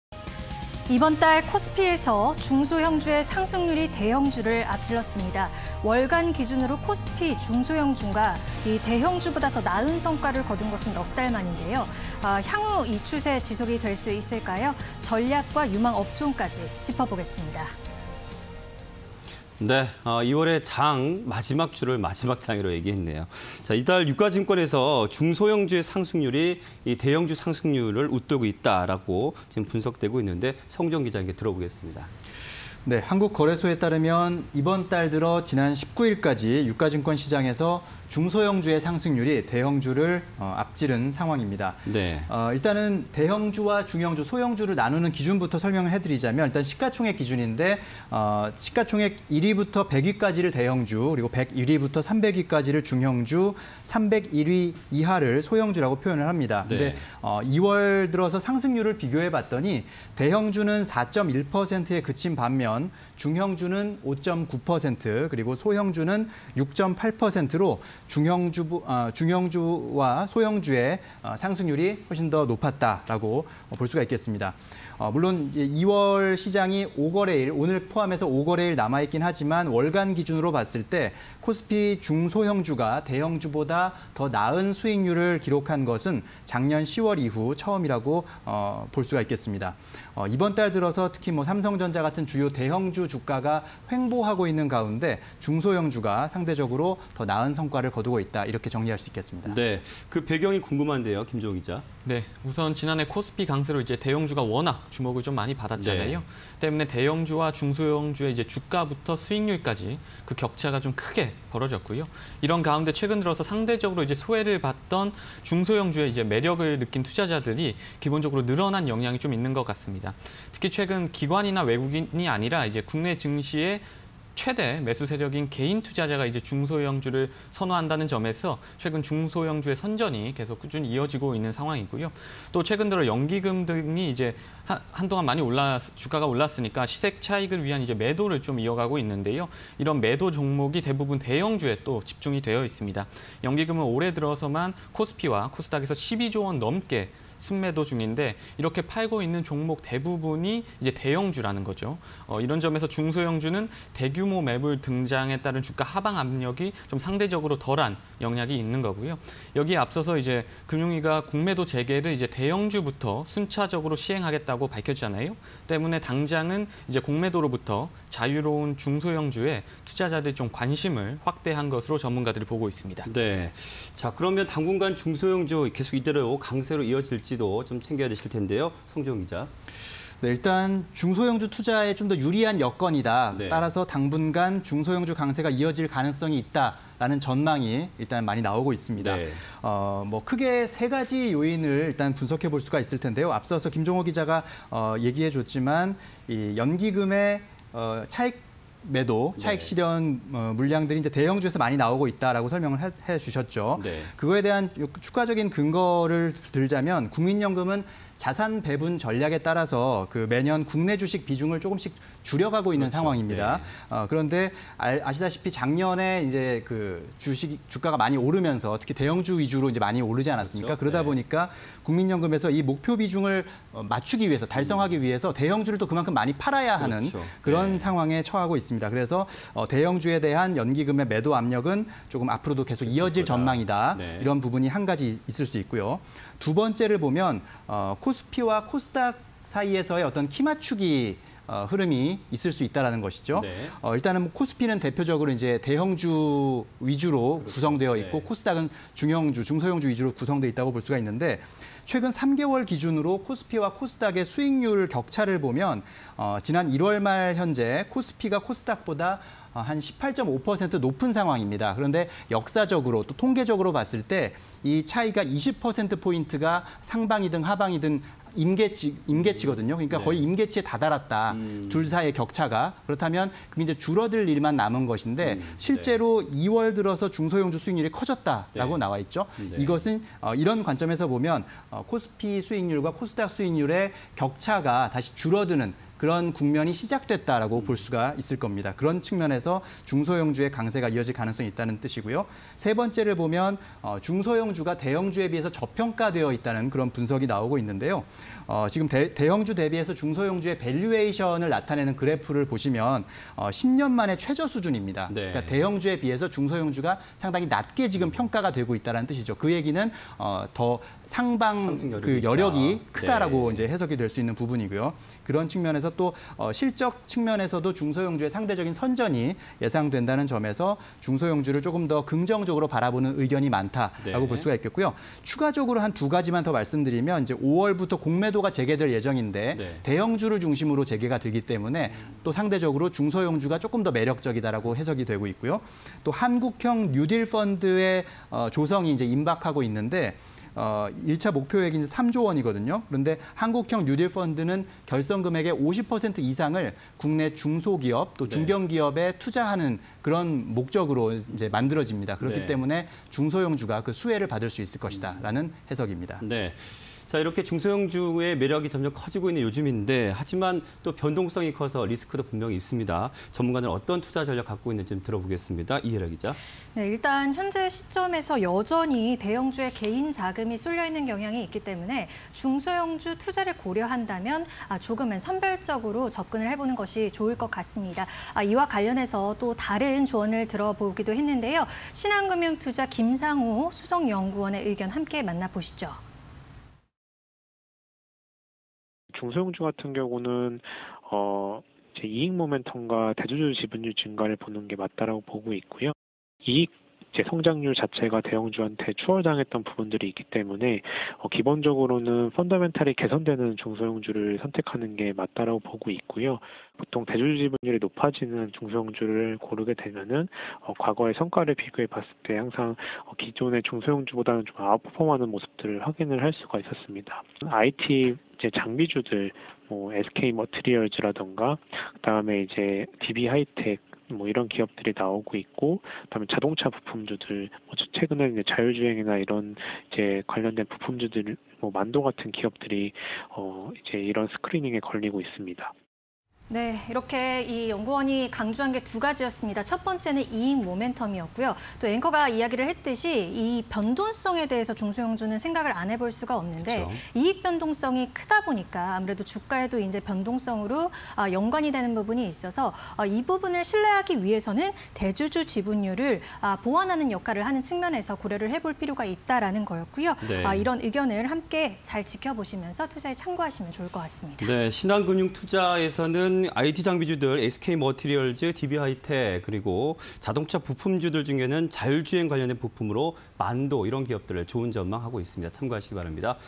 개별주 장세 오나… 중소형주, 대형주 상승률 웃돌아_뉴스IN이슈 (20210218)
- 소외받던 `중소형주`에 매력 느낀 투자자 늘며 선전
- 연기금 등 시세 차익 매도 `대형주` 집중... 하방 압력 덜해
- 대형주 `공매도 재개` 임박... 중소형주 관심 커져
- 5월 대형주 중심 공매도 재개 예정
- 한국형 뉴딜펀드 조성 수혜… 50% 이상 中企 투자
- 중소형주 선별적 투자 전략 유효